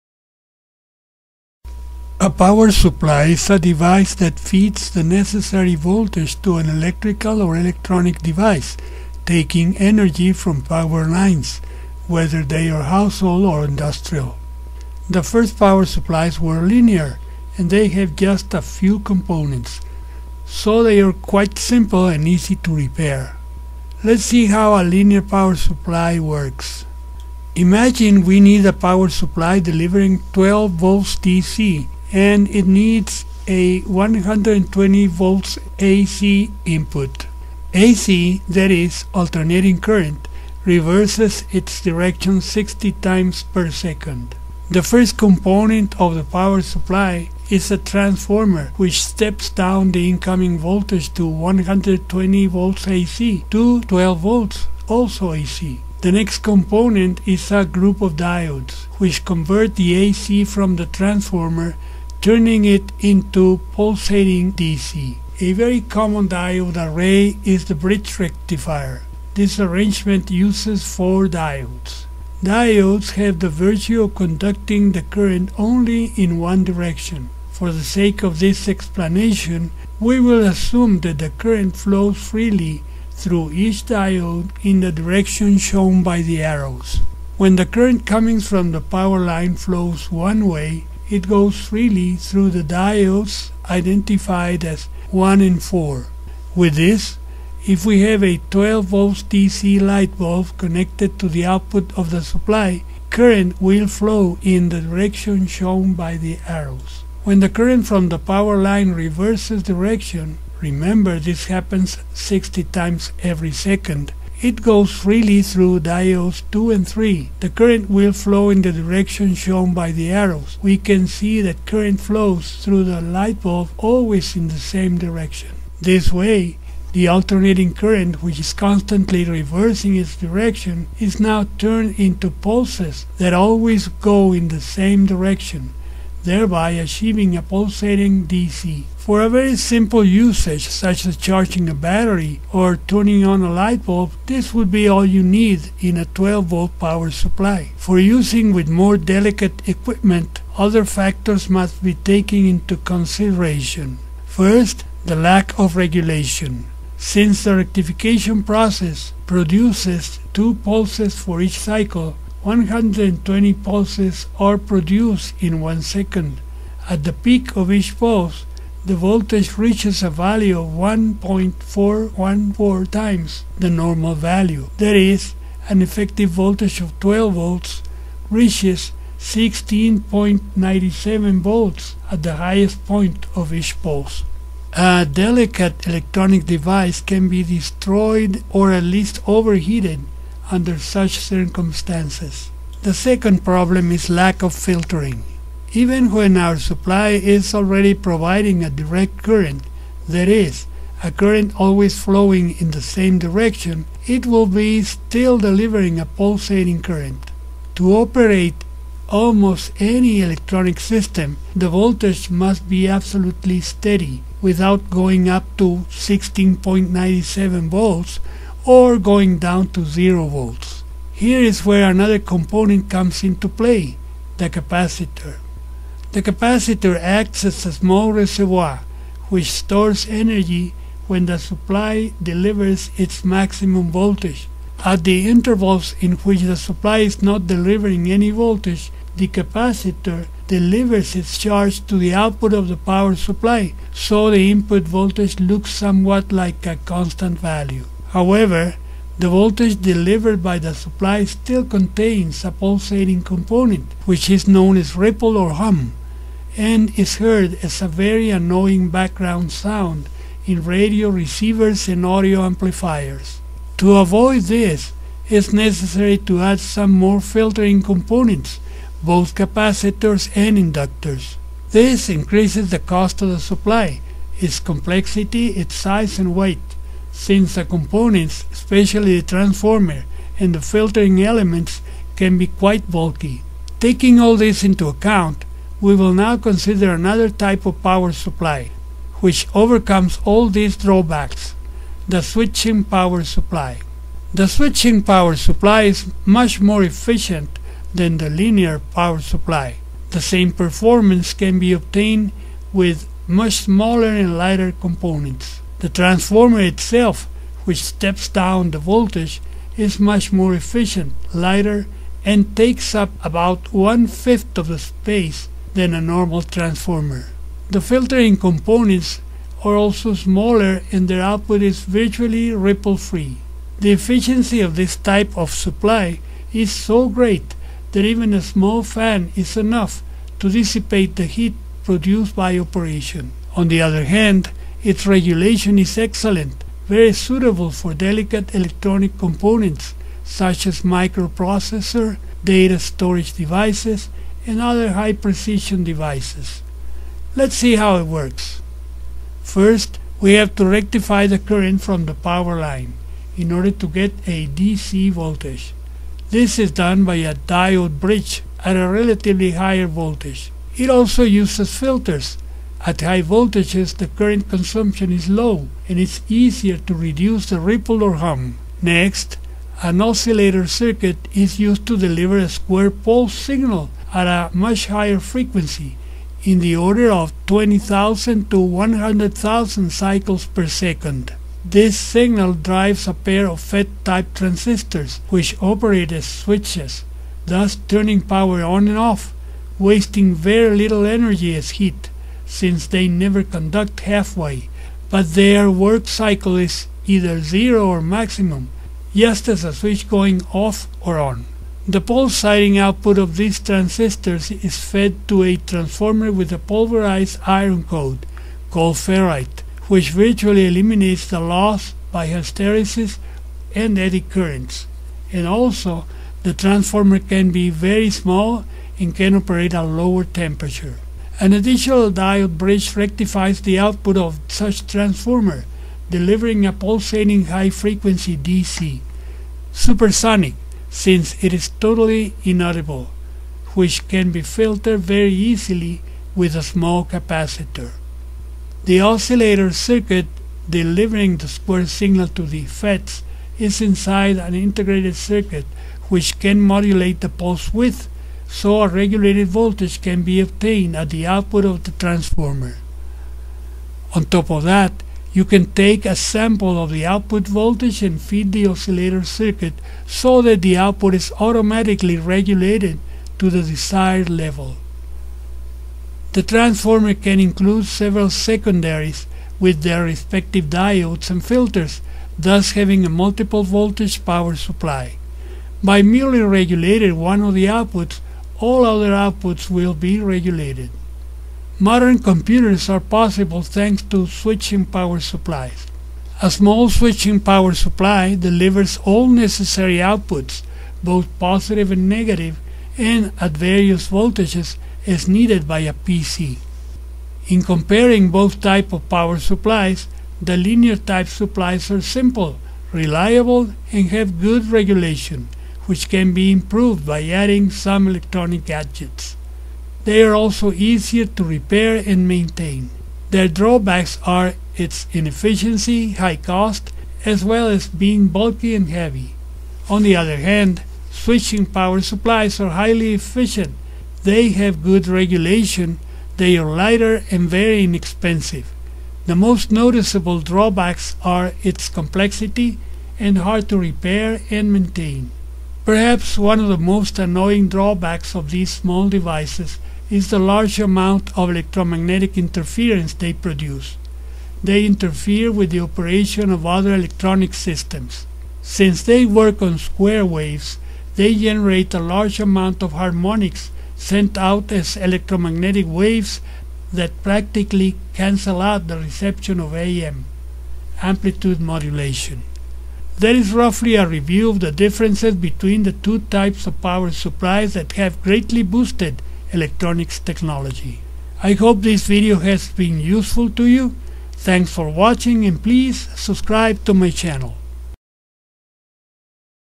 Basic differences between linear and switching power supplies
Very basic description of two main types of power supplies. Advantages and drawbacks.